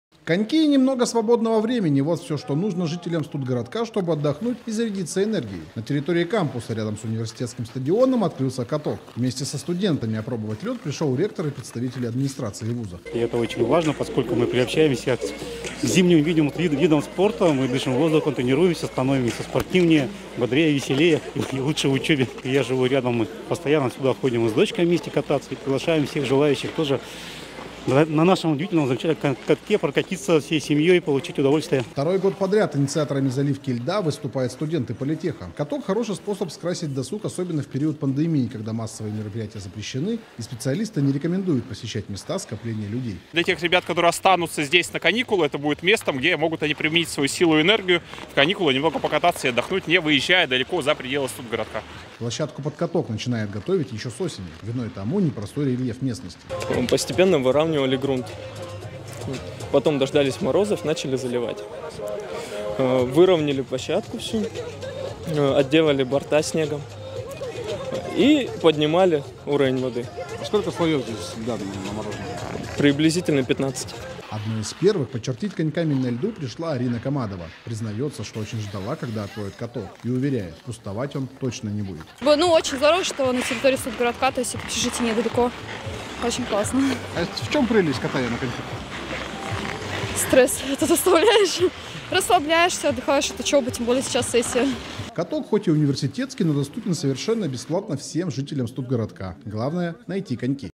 Каток в студгородке
На территории кампуса, рядом с университетским стадионом открылся каток.